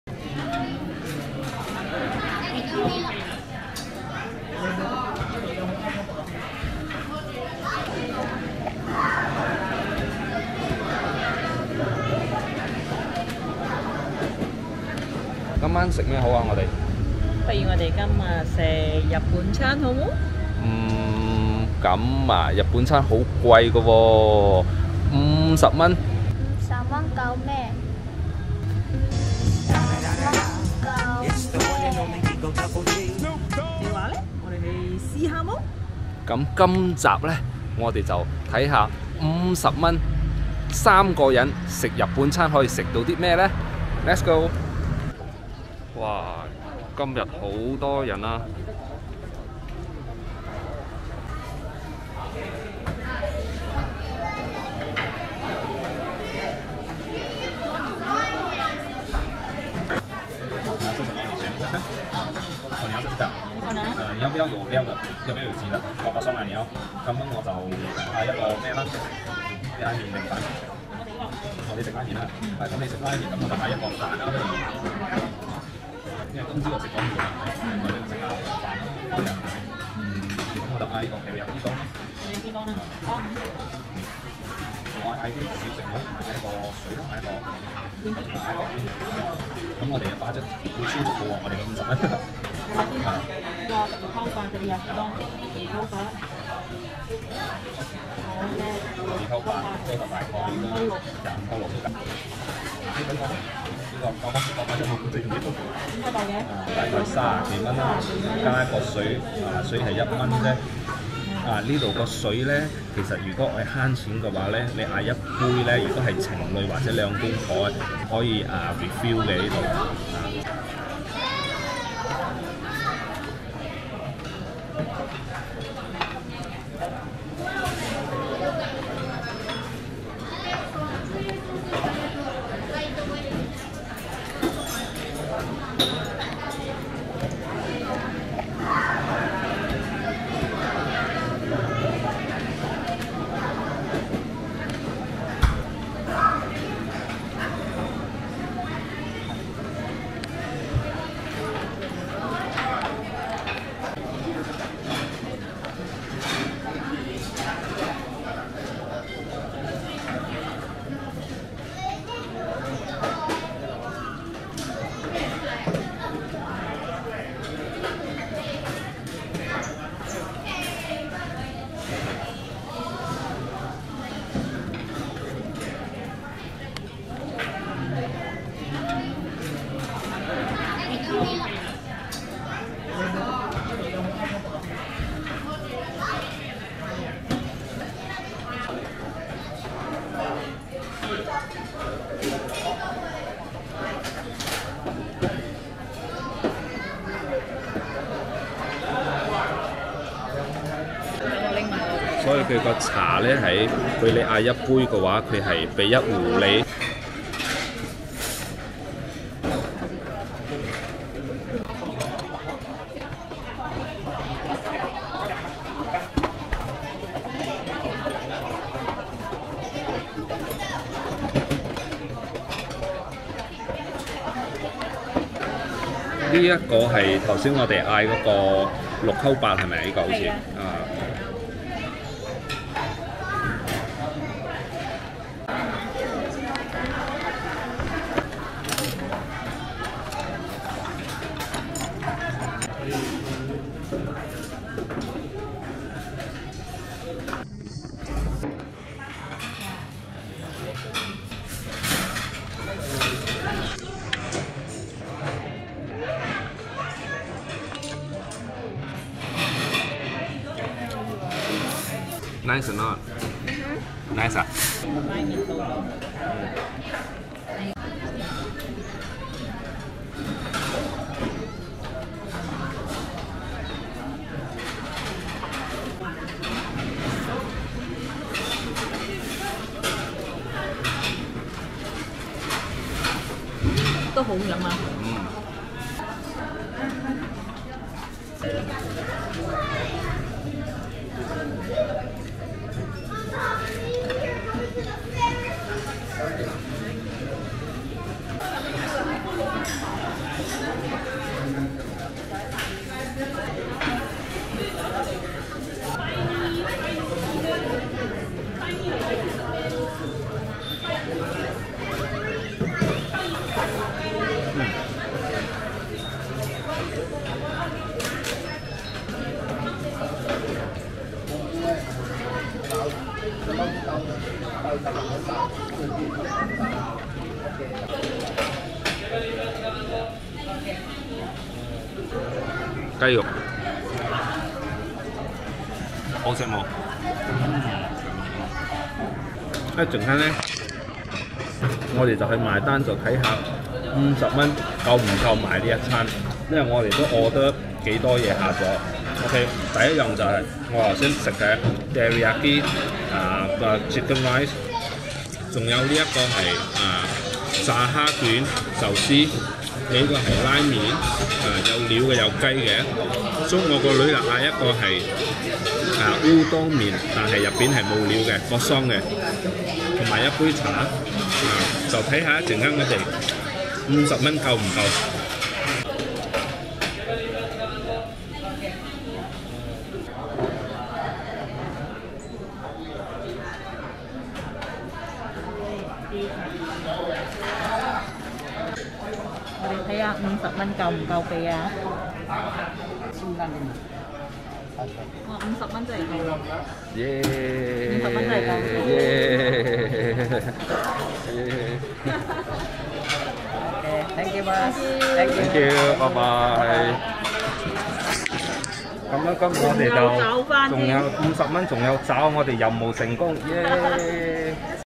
最便宜的日本餐 | Cheapest Japanese Meal
In this episode, our dinner budget only RM50 / $11 USD for having Japanese meal for whole family 3 persons.

Filmed: 18 Sept 2022